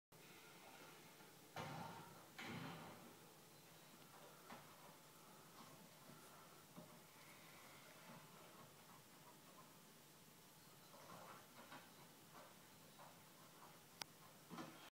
Baby min pin Annie discovering a slipper!
Little miss Oakley having fun 10 May 2012